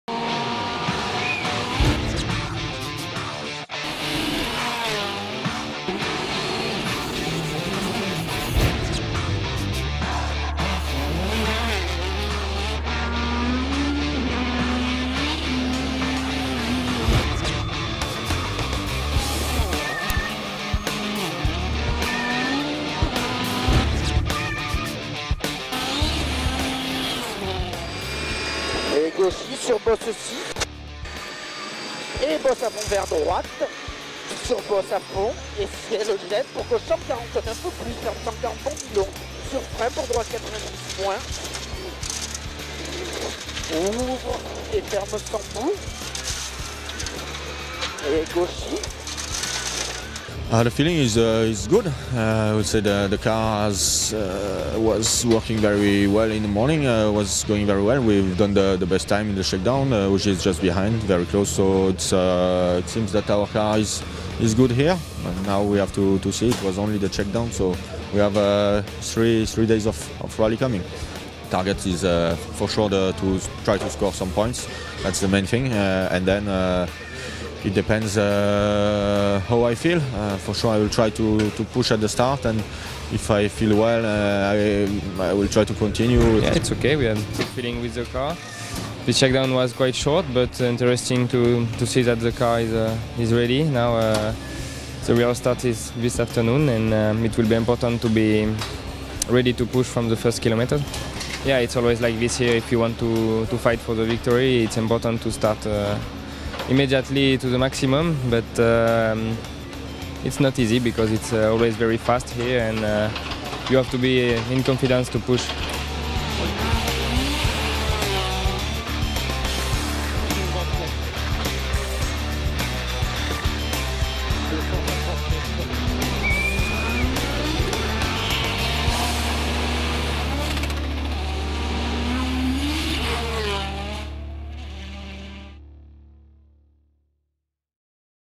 Citroën Racing - WRC 2011 - Rally Finland - Shakedown
Rally Finland begins today after three days' reconnaissance. The Citroën DS3 WRCs took part in the shakedown this morning to prepare for the 314 kms to come. Sébastien Loeb and Sébastien Ogier laid down a marker by setting the two fastest times!